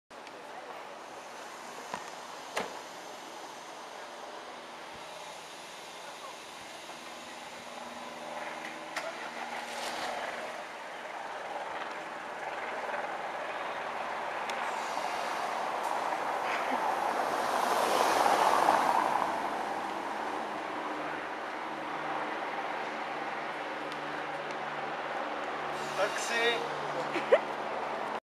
Modern beggar
Just a short sketch about that you don't even need to look like beggar to beg some money for your bussines